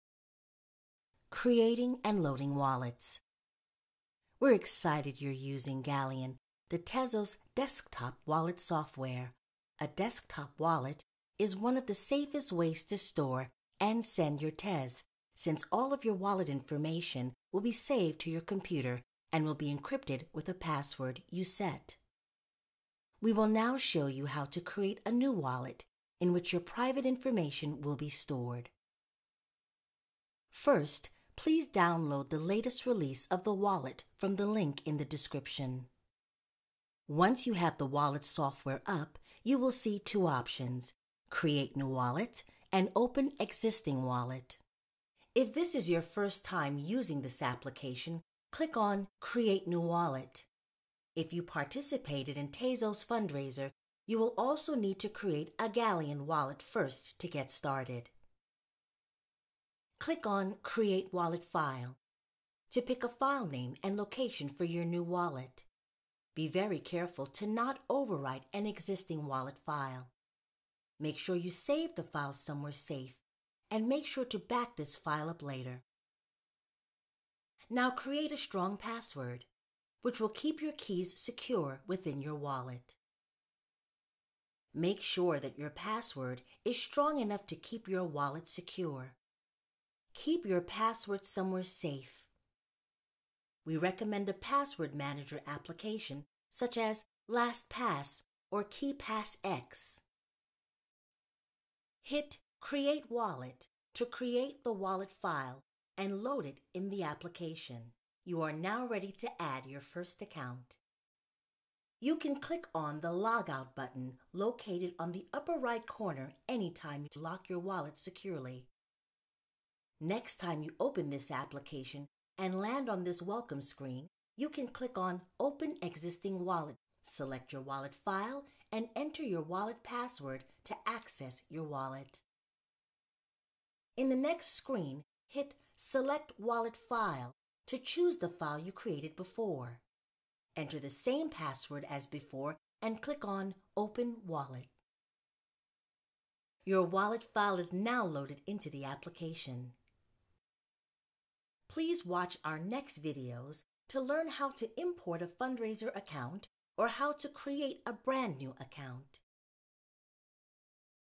Galleon Tutorial: Creating and loading wallets
A short tutorial on how to create a brand new  Tezos wallet using Galleon.